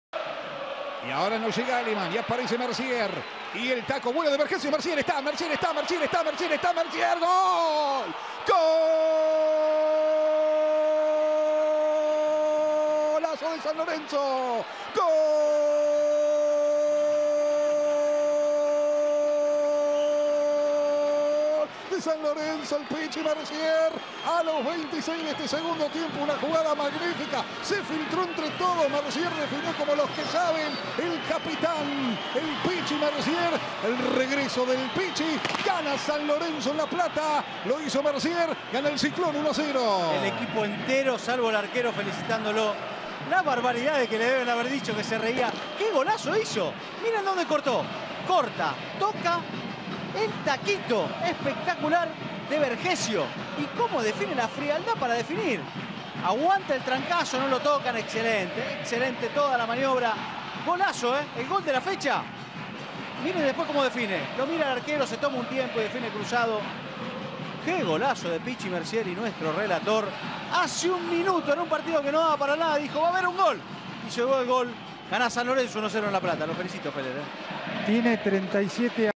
Goles de San lorenzo de Almagro 2017
El Pichi Mercier pone el 1 a 0 contra gimnasia de la plata